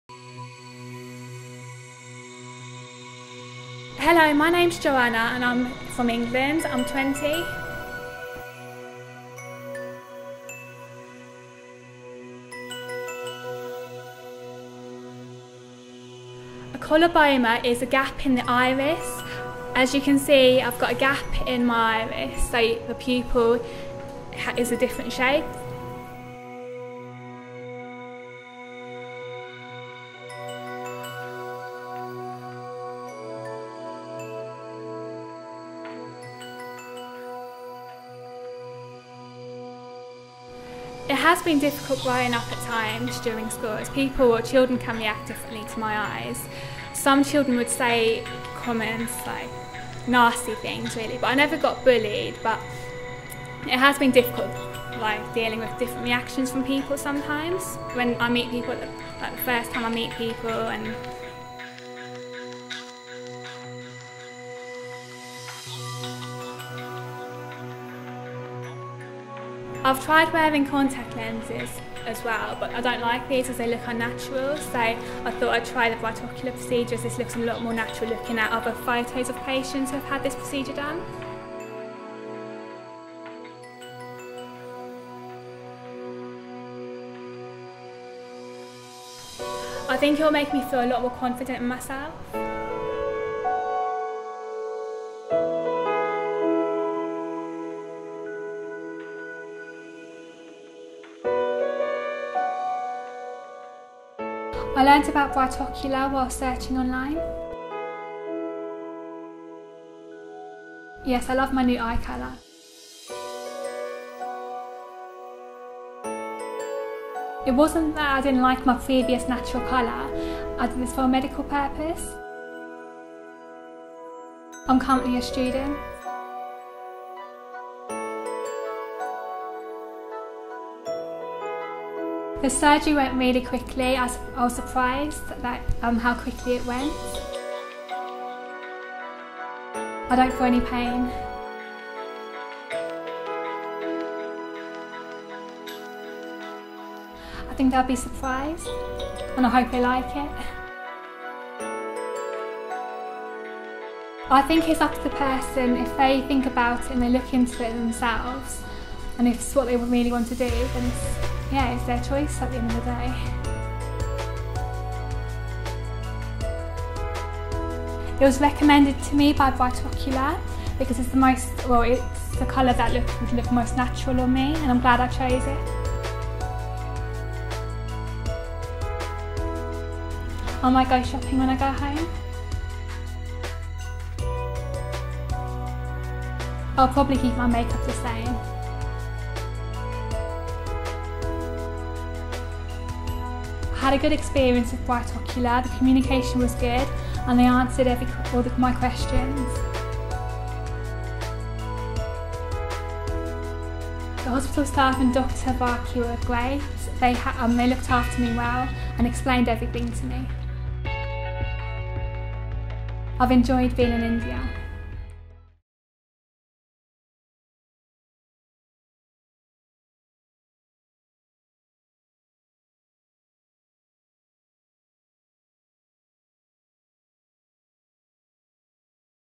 Artificial iris treats patients iris coloboma / brightocular
Artificial iris implant corrects the iris coloboma and photo sensitivity.